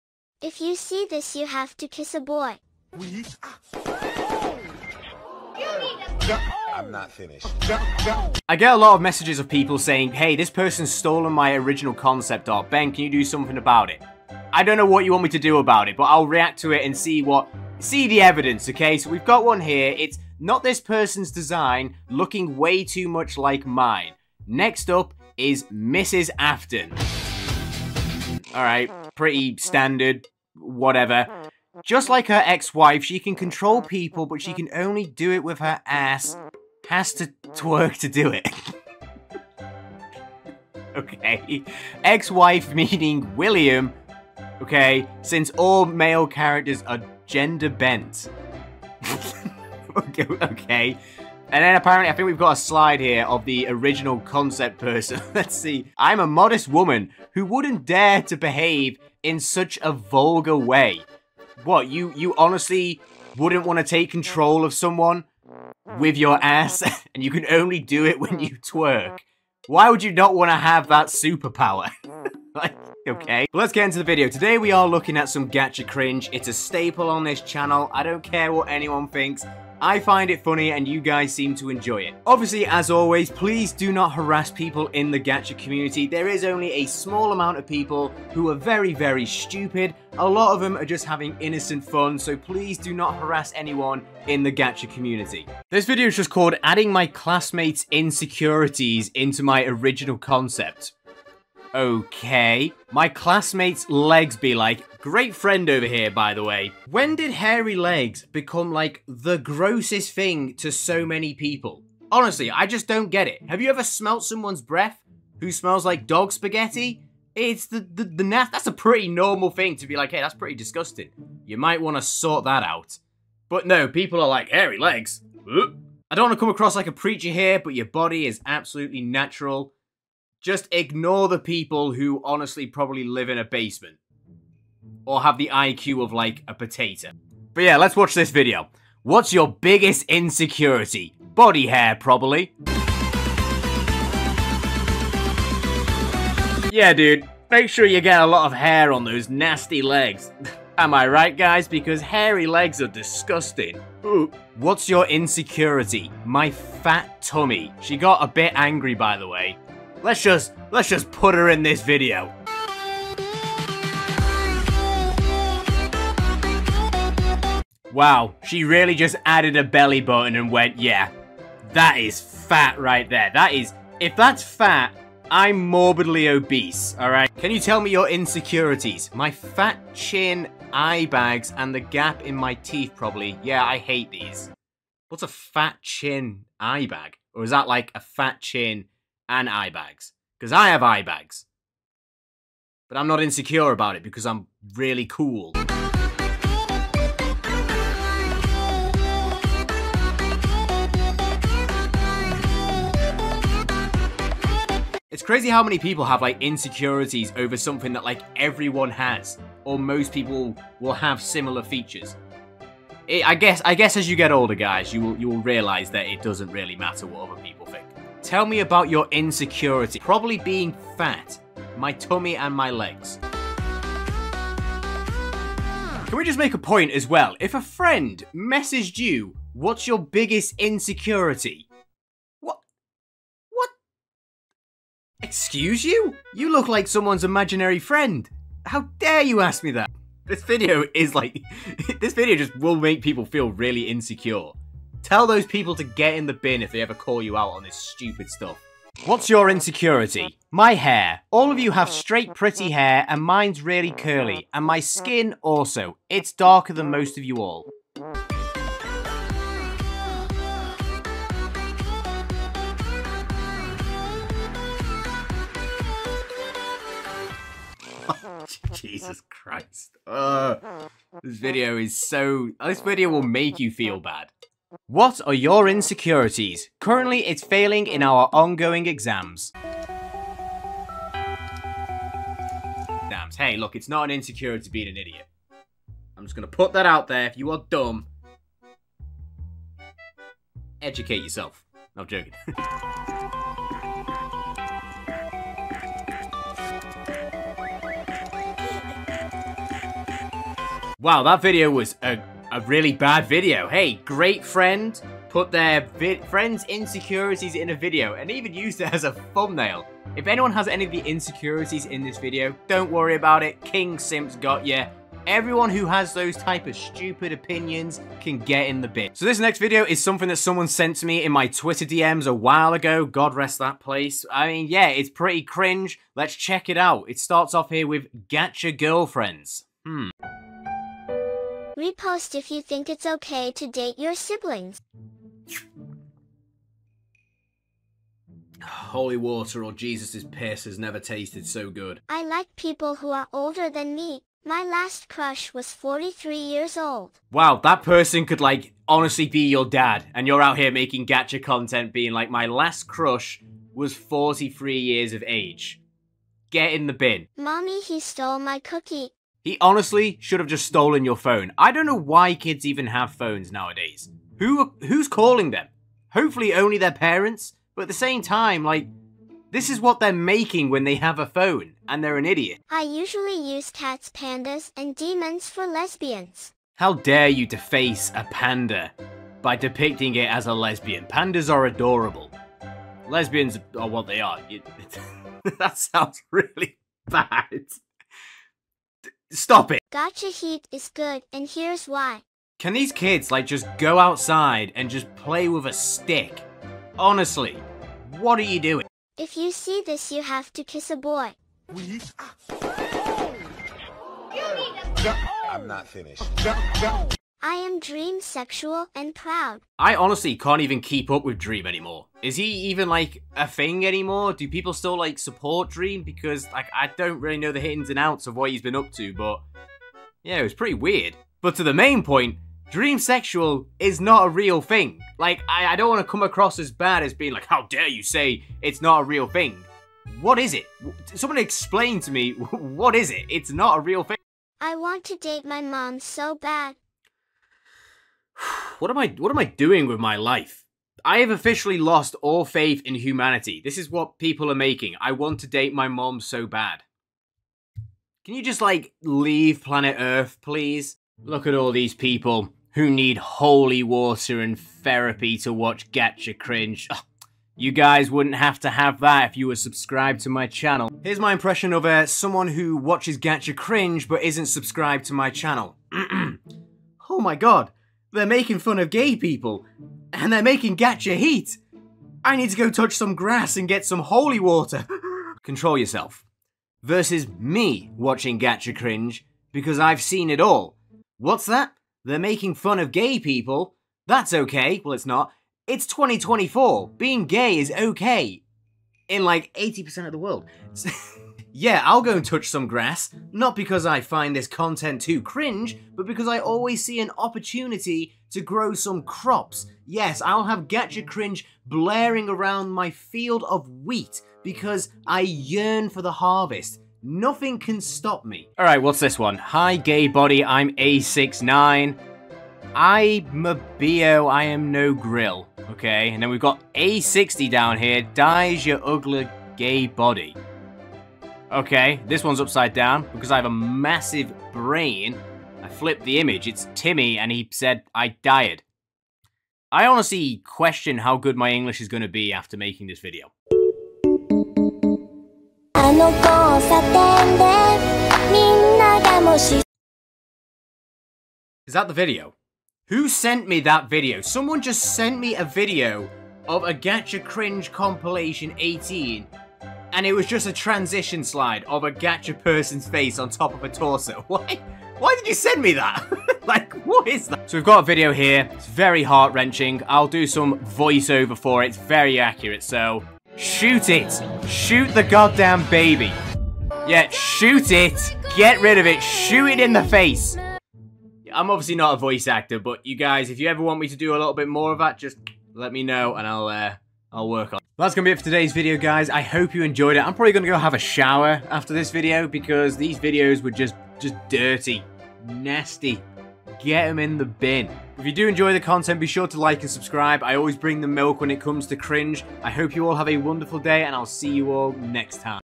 Reacting to Gacha Life Cringe 💀 THIS IS NOT OK!
Today were reacting to gacha cringe, gacha cringe edits are something to be cringe at but gacha life cringe is a whole new level